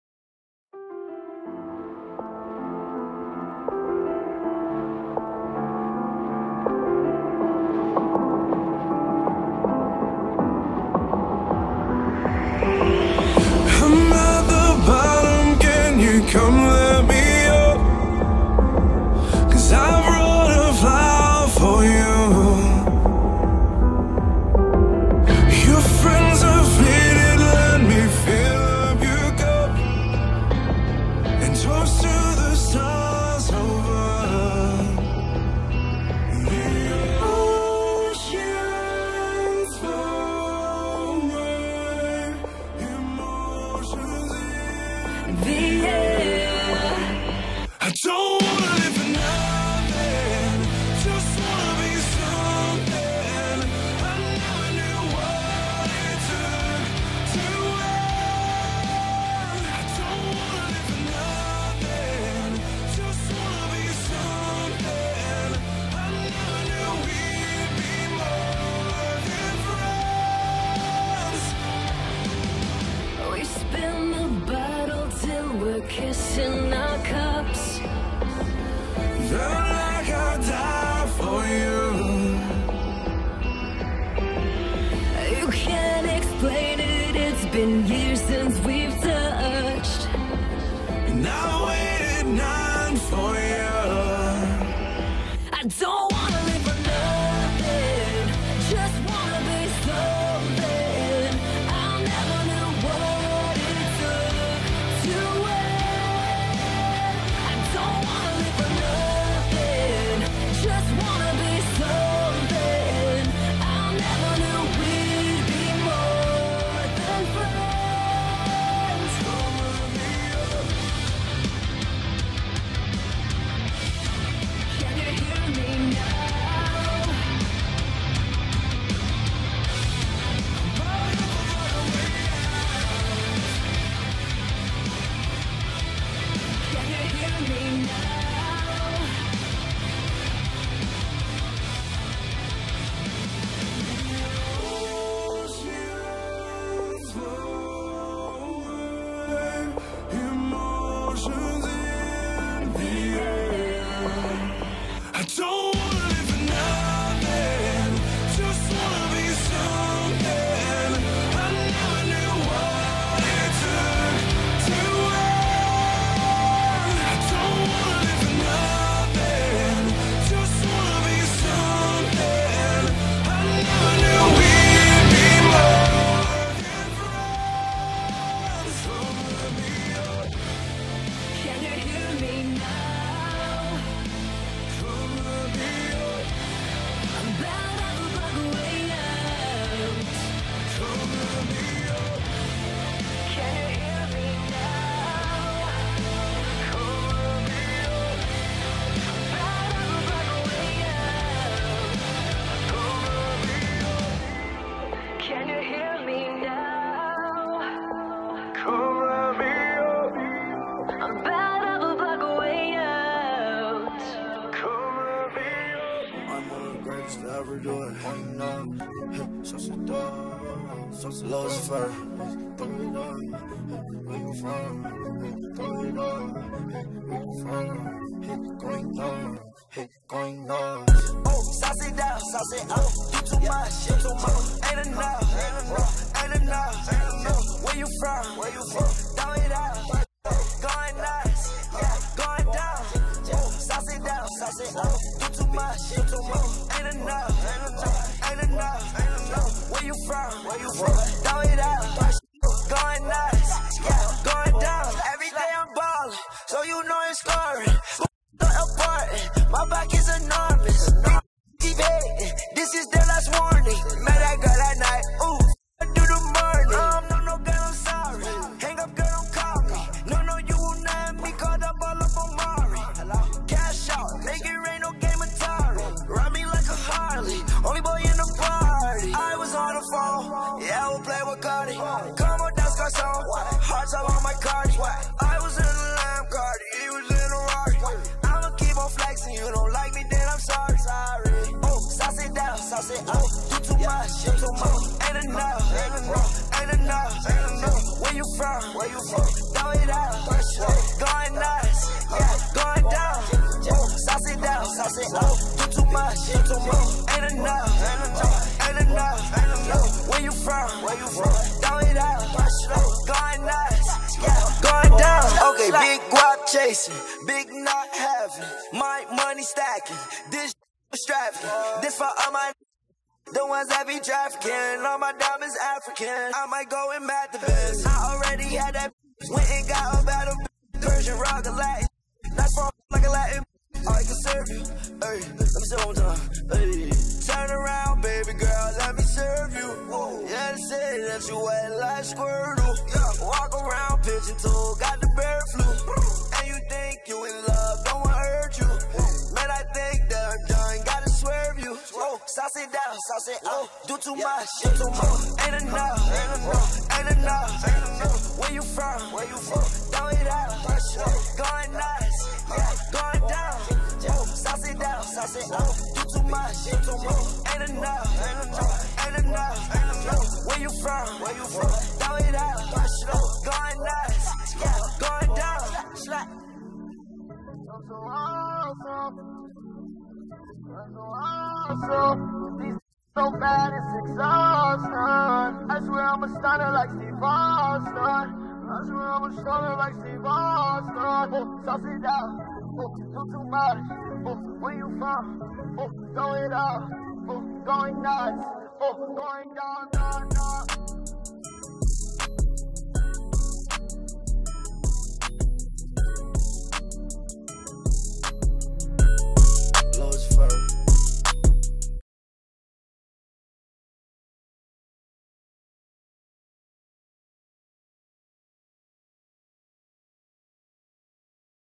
Pikeville vs Raceland 2019 Week 4 Highlights
The Panthers hang on to defeat the Raceland Rams in the 2019 Hall of Fame Game.

Re-upped to fix music error.